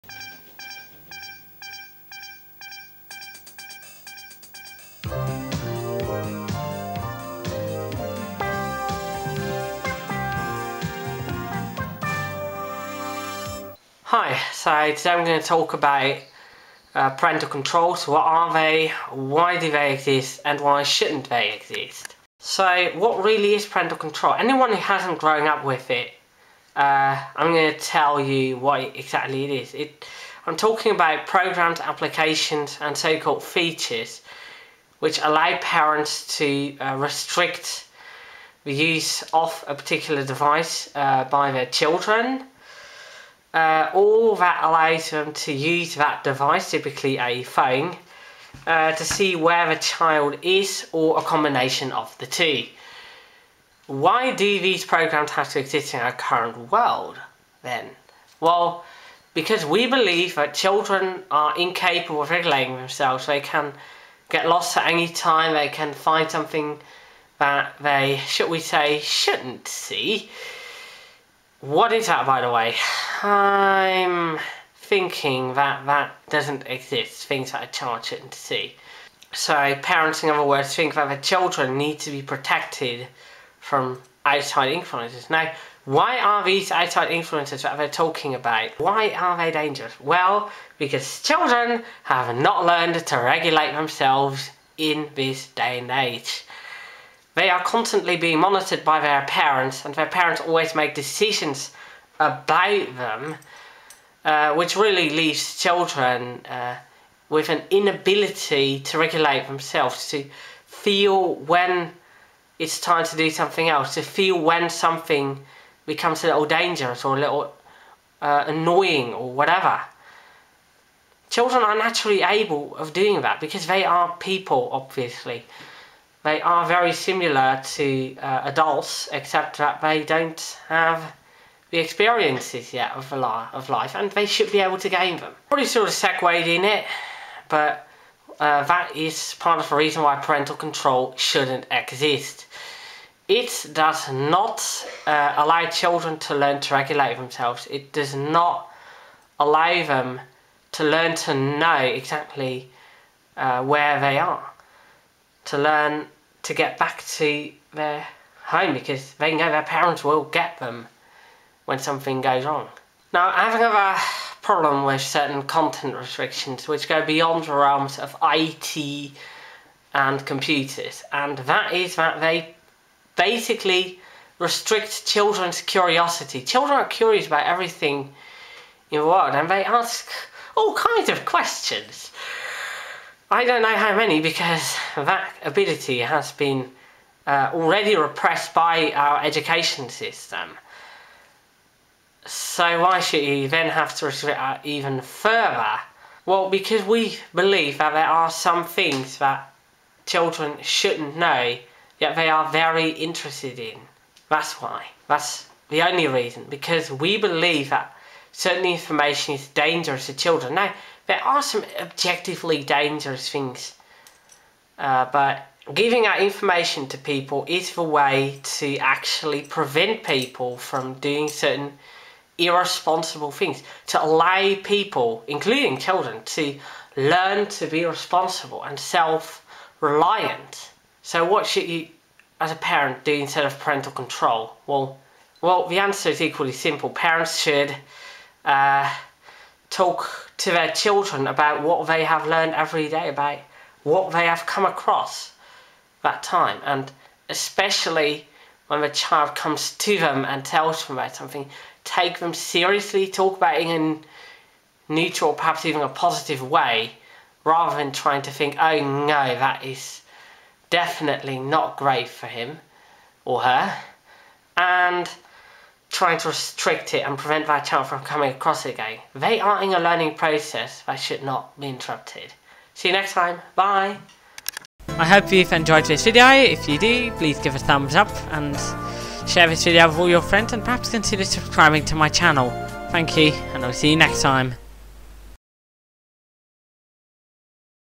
Parental controls: Are they any good?
Today let's talk about something I didn't grow up with, but many from the 2000s did: Parental controls. Restrictions of children's curiosity and movement.